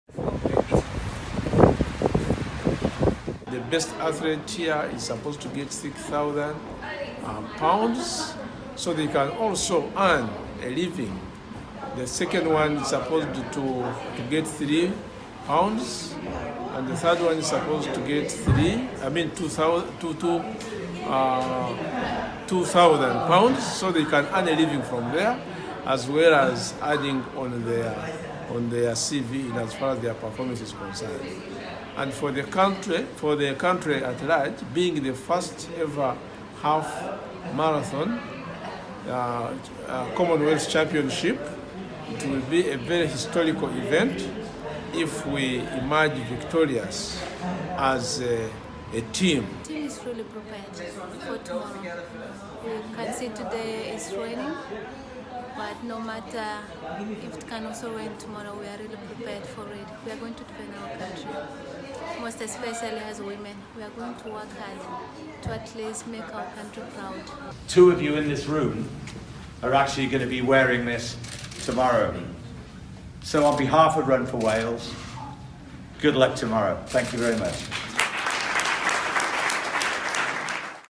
Team Uganda arrives in cardiff for Commonwealth half Marathon
The Uganda long distance team has arrived in Cardiff, United Kingdom, where they will participate in the inaugural commonwealth half marathon on Sunday. The team, which comprises four male and three male athletes includes Fred Musobo, Felix Chemonges, Moses Kwemoi, Timothy Toroitich Lynette Chebet, Juliet Chekwel and Emilly Chebet.